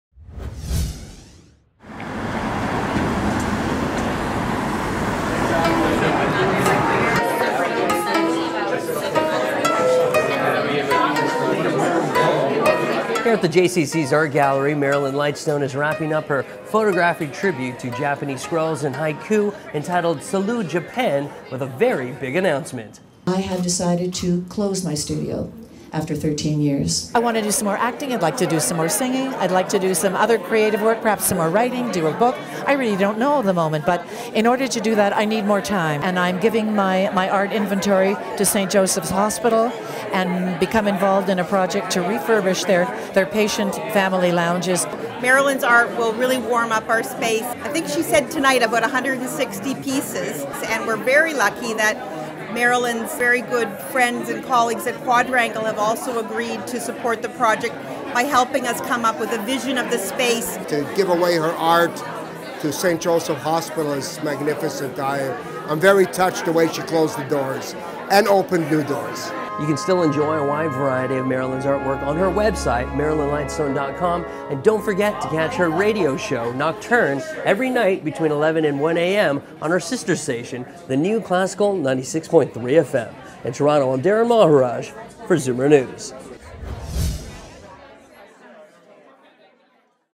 Znews - Marilyn's Lightstone Donates Artwork to St. Joseph's Hospital
Znews attends the closing reception of "Salut! Japan"  Marilyn Lightstone's photographic tribute to the traditional Japanese art forms of scrolls and haiku. At the event the well known actress, radio host and artist  made a very special announcement...